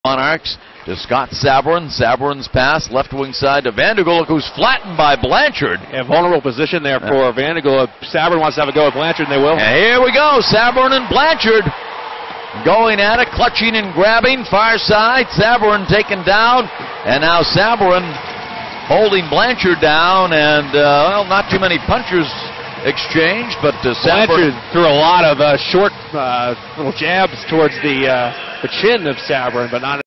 Nicolas Blanchard vs Scott Sabourin Mar 21, 2015
Nicolas Blanchard vs Scott Sabourin from the St. John's IceCaps at Manchester Monarchs game on Mar 21, 2015.